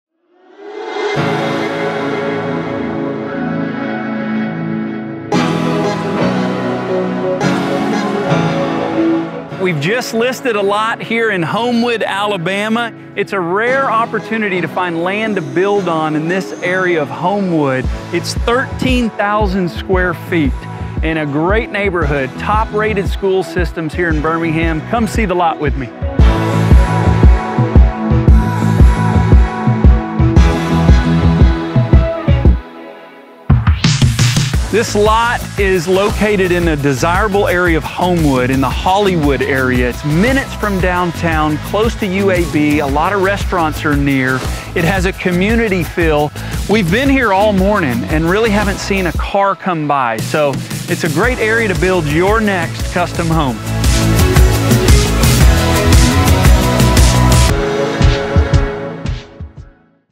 510 Tamworth Lane, Homewood, AL 35209 | Lot for Sale | David Ridley | Southern State Realty Partners
Presented by David Ridley and Southern State Realty Partners

This is a rare opportunity in Hollywood to build on a lot with privacy in a desired area of Homewood with easy access to HW 280. Close to Shades Cahaba Elementary and Brookwood Mall.